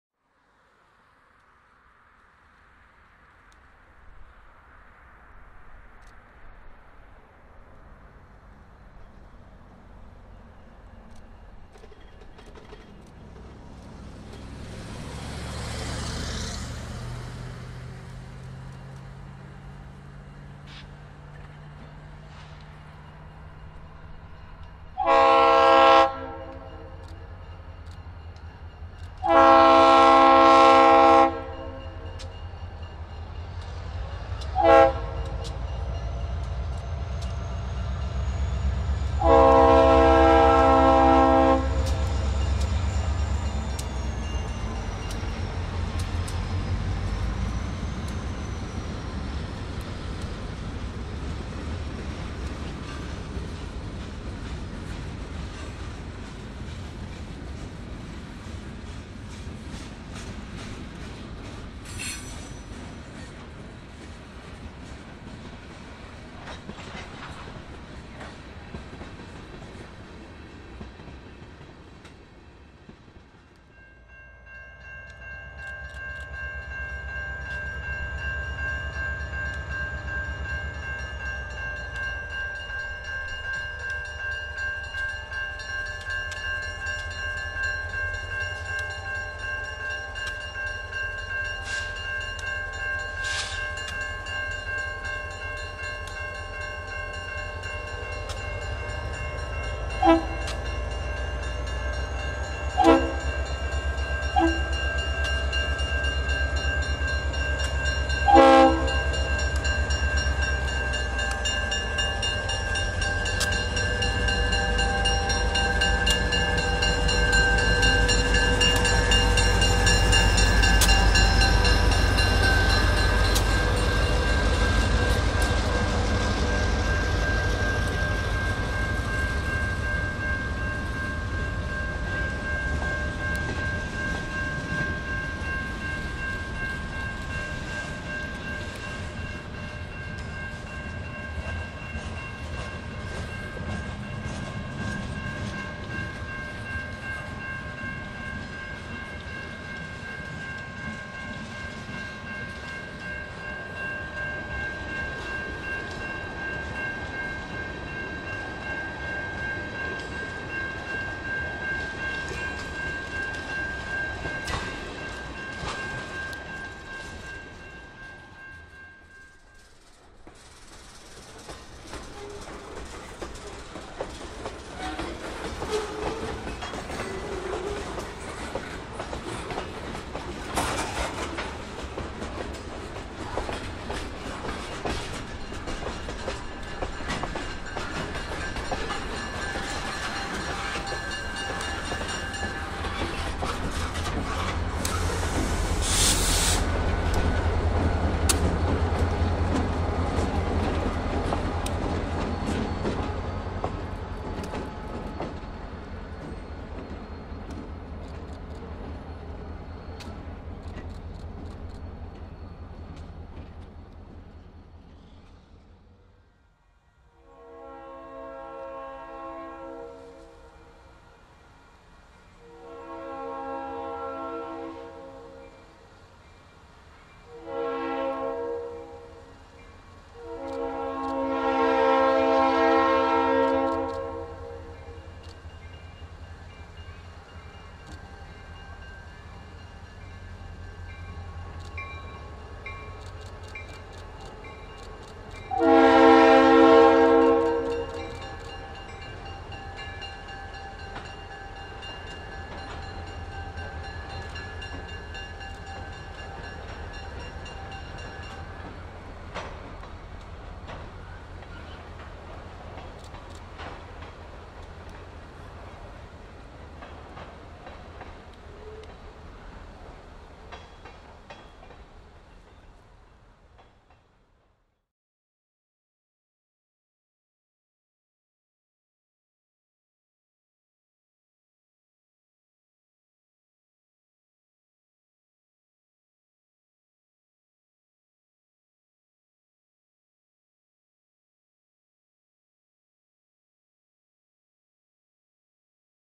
Mass Coastal MC-4 on the Dean St. Industrial Track - 3/24/2023
In this video we see Mass Coastal MC-4 running on the Fall River Secondary then making a move on the Dean St. Industrial Track in Taunton, MA.

South Coast, MA
3/24/2023

Comments? Questions?